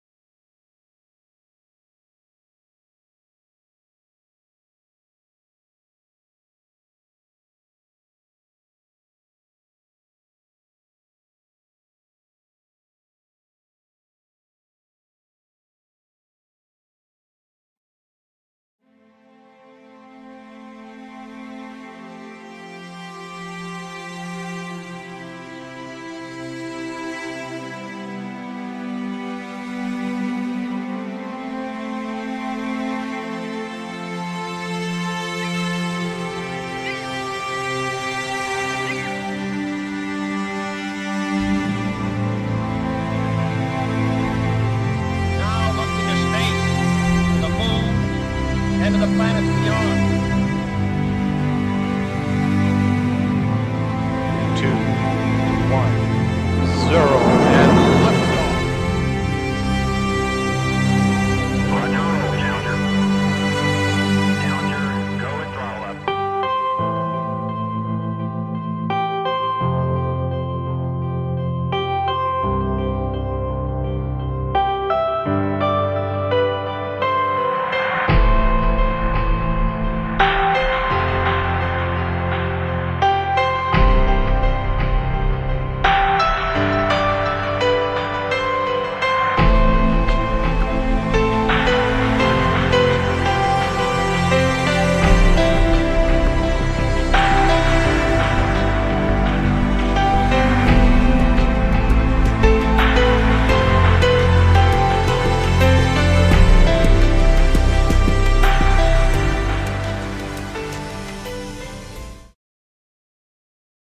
CHRISTA: Where the Heart is (Trailer)
Full Release of Video on 2/12/2013 as a Valentines Day Tribute to America's Sweetheart Teacher.

Tribute to a Mother, Teacher, Astronaut, and Medal of Honor Winner!